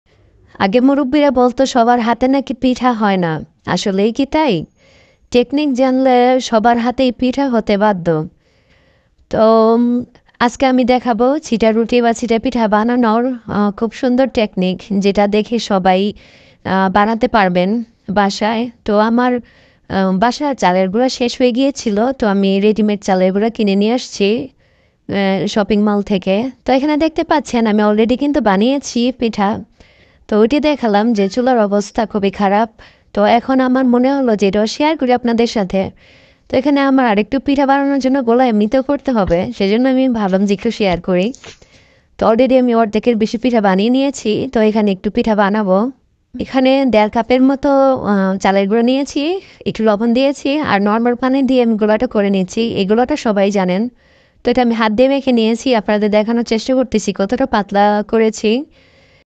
ছিটা রুটি বা পিঠা (Part 1)এই technique এ বানান & সবাই কে অবাক করে দিন।ছিটা পিঠা সহজ রেসিপিbanglades
ছিটা রুটি বা পিঠা এভাবে বানান-যে কেউ পারবেন & অর্ধেক ঝামেলা কমে যাবে ১০০%।হে এভাবে ও দারুণ পিঠা হয়😉

ছিটা রুটি রেসিপি
ছিটা পিঠা রেসিপি
Bangladeshi mom in dubai with two kids Ayaan & Adhiyaan

Sita pitha recipe
Sita ruti recipe

How to make easy sita pita.